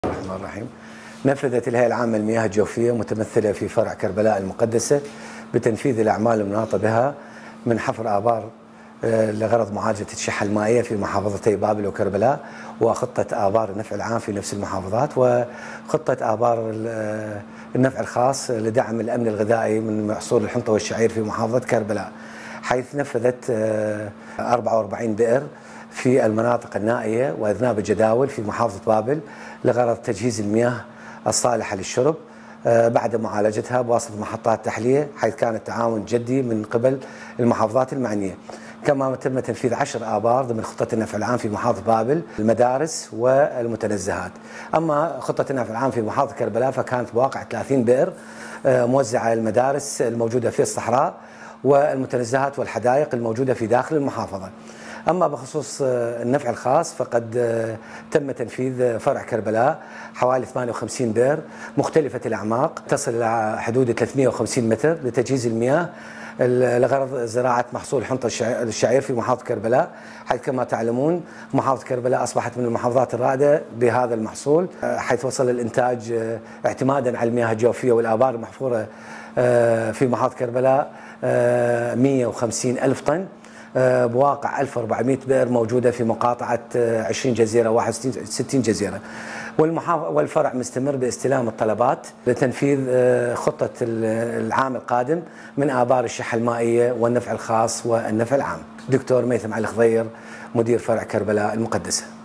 الهيأة العامة للمياه الجوفية فرع كربلاء المقدسة تنفذ خطتها لحفر الآبار المائية ضمن العام الحالي
الهيأة العامة للمياه الجوفية فرع كربلاء المقدسة تنفذ خطتها لحفر الآبار المائية ضمن العام الحالي خطتي الطوارئ ومعالجة الشحة المائية